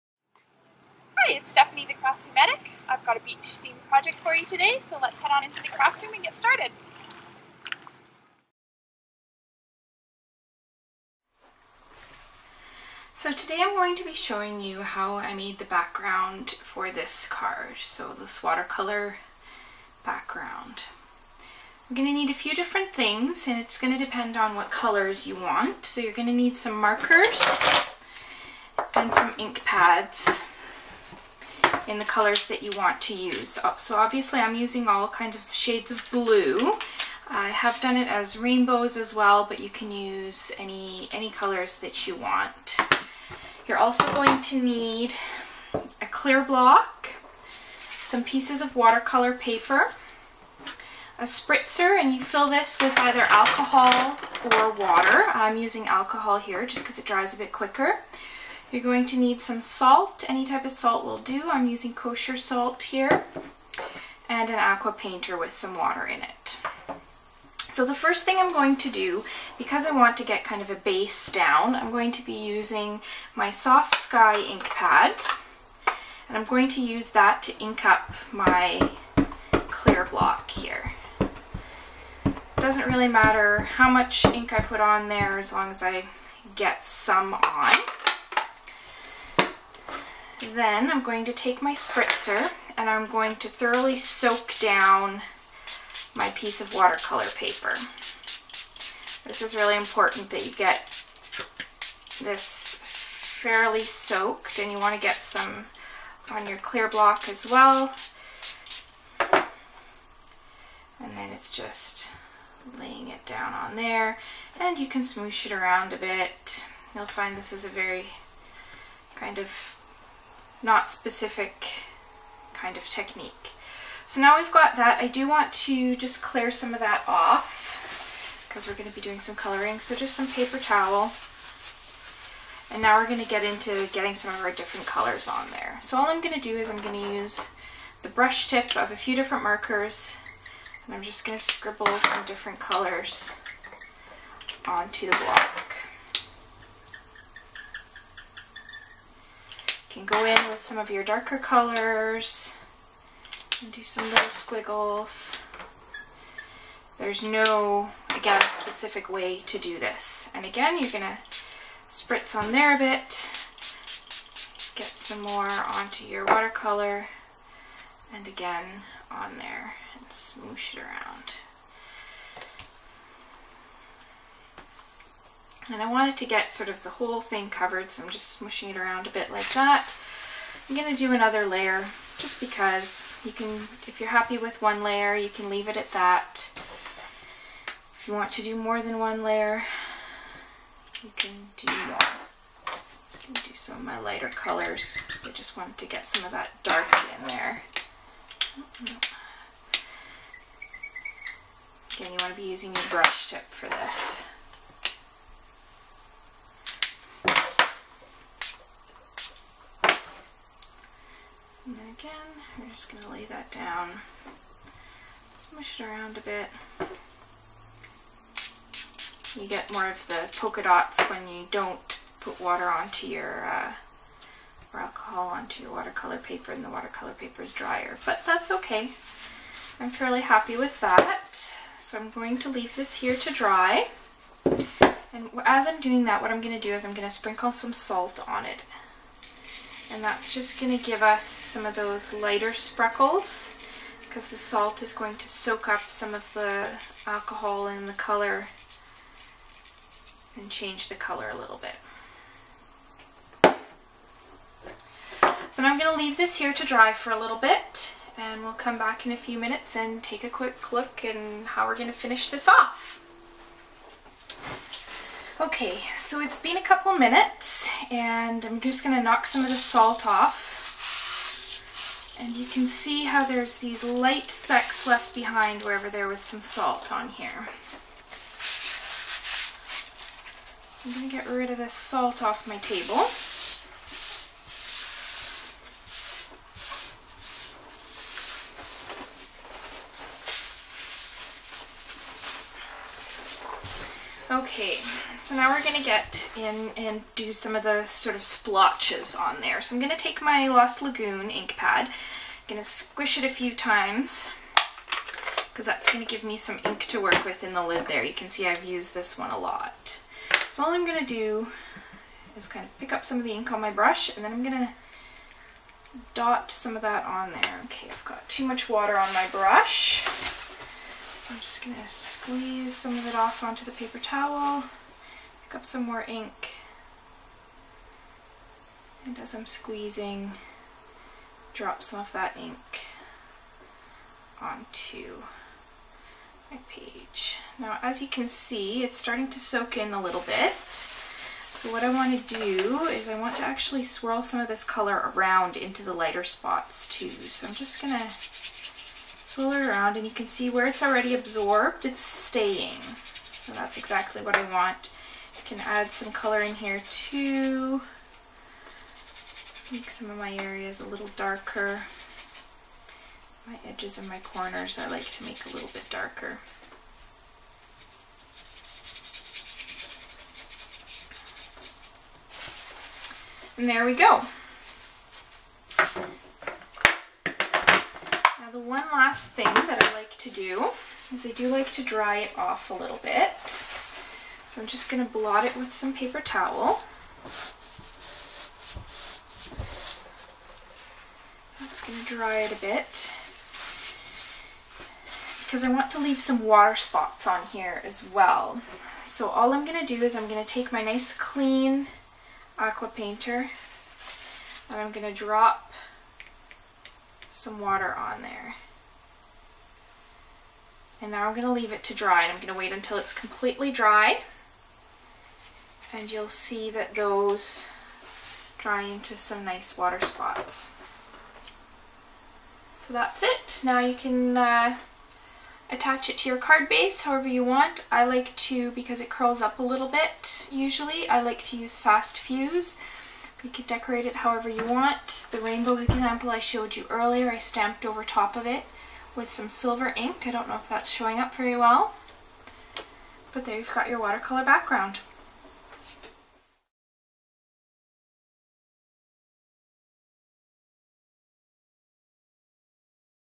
Watercolor Backgrounds with The Crafty Medic
Learn how to make these fabulous watercolor backgrounds with Stampin' Up! tools and supplies.